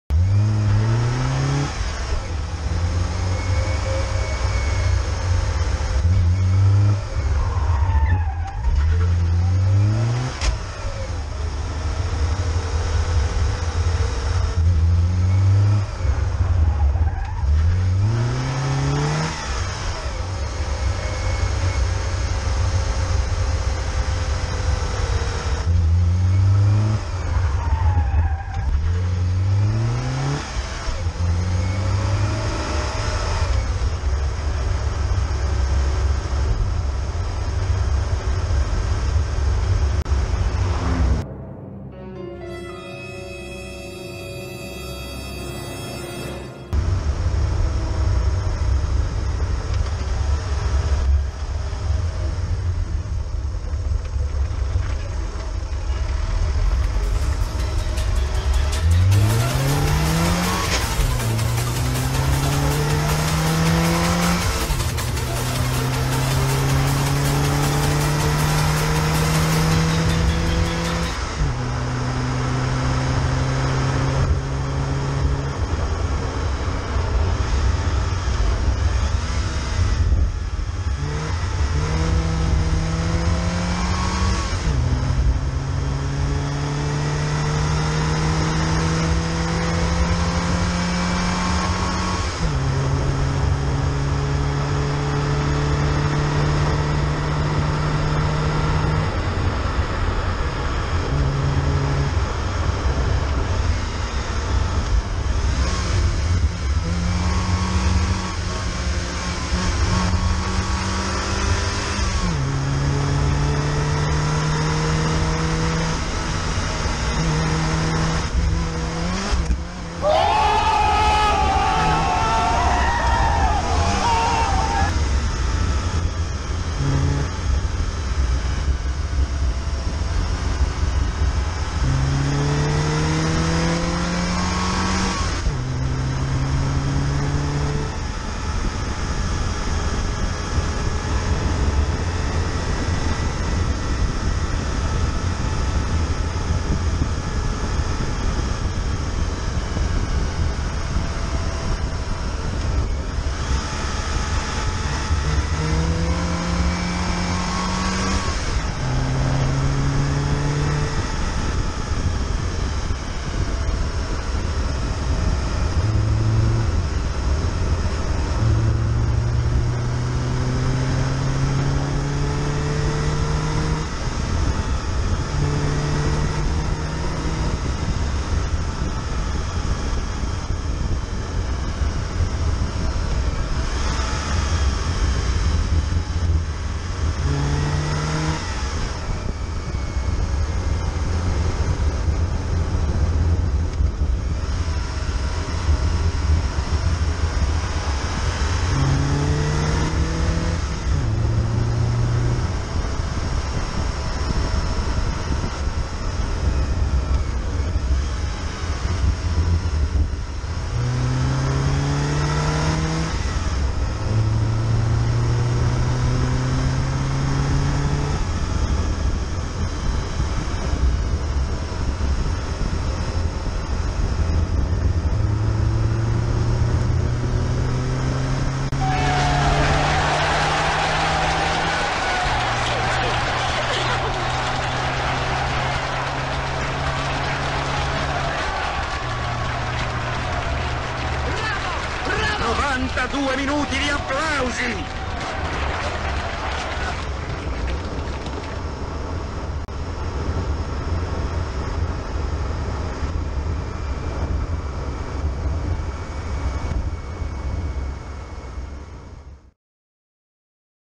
Quando hai una Panda ed incroci una 500 Abarth! 🙃
Video puramente ironico, in questo video non è stata maltrattata nessuna tartaruga.

P.s. la panda è ufficialmente tornata!